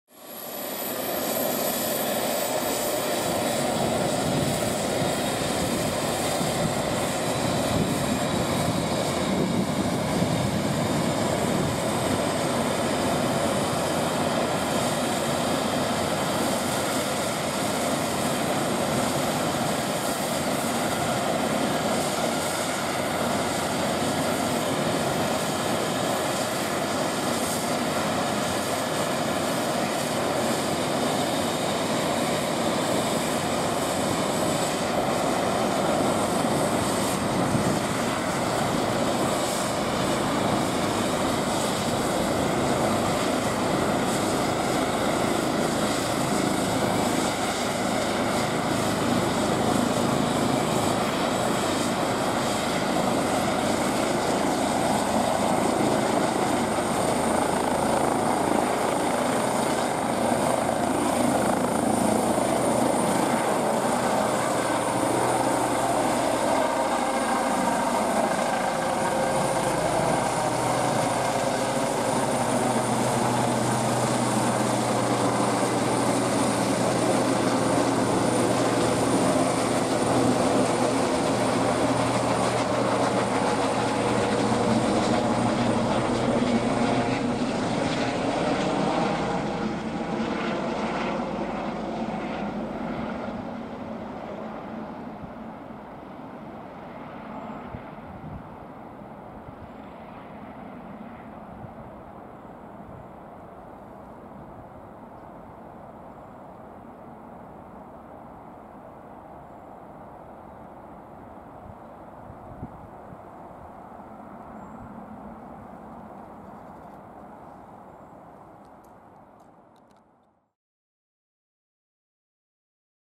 Christoph 9 Hubschrauber Start am HELIOS Klinikum Wuppertal Barmen
Wir haben den Hubschrauber D-HZSN vom Christoph 9 aus Duisburg am HEIOS Klinikum Wuppertal Barmen gefilmt.